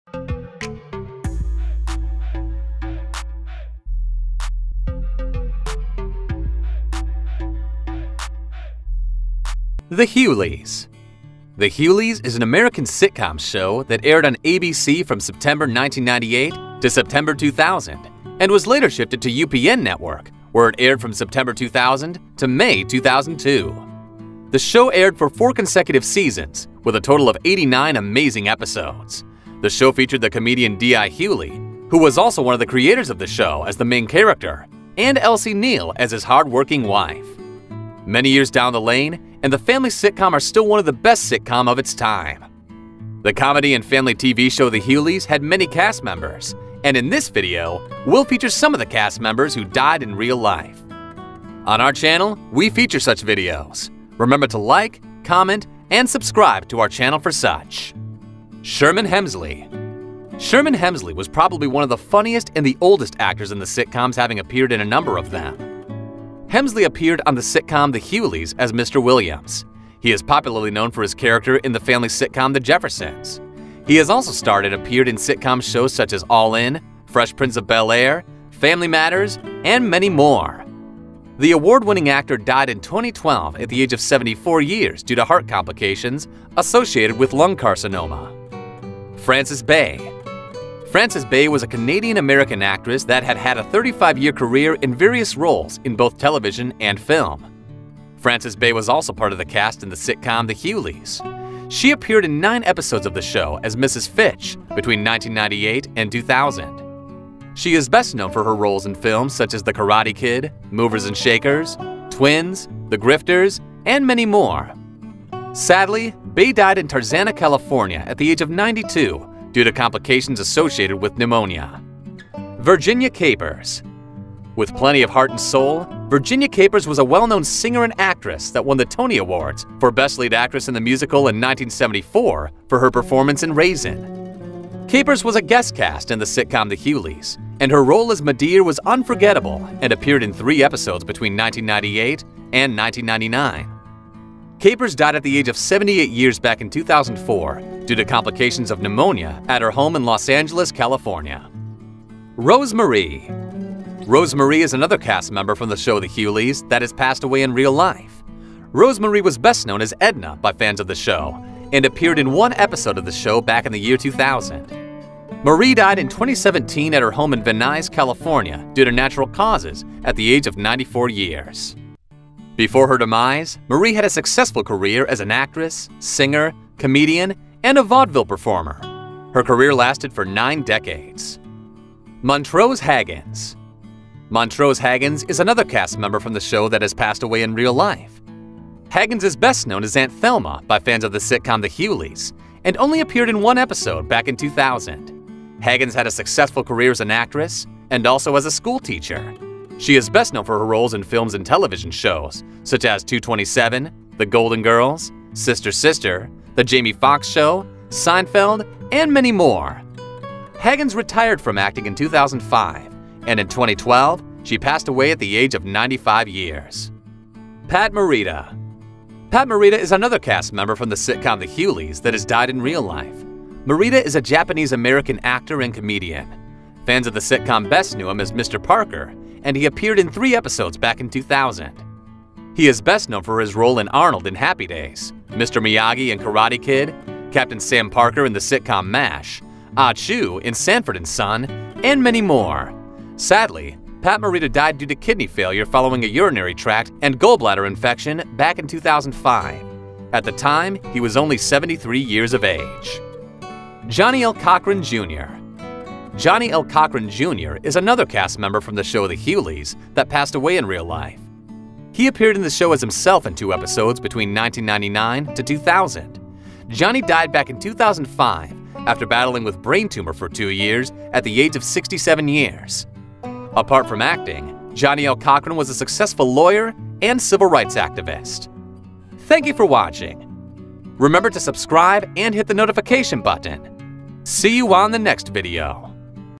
7 Actors From THE HUGHLEYS Who Have Died
The Hughleys is an American sitcom show that aired on ABC from September 1998 to September 2000 and was later shifted to UPN Network where it aired from September 2000 to May 2002. The show aired for 4 consecutive season with a total of 89 amazing episodes. The show featured the comedian D.L Hughley who was also one of the creators of the show as the main character and Elsie Neal as his hard-working wife. Many years down the lane and the family sitcom are still one of the best sitcom of its time. The comedy and family tv show, The Hughley had many cast members and, in this video, will feature some of the cast members who died in real life such as Sherman Hemsley, Frances Bay, Rose Marie, Montrose Hagins, Pat Morita, Virginia Capers, and Johnnie L. Cochran Jr.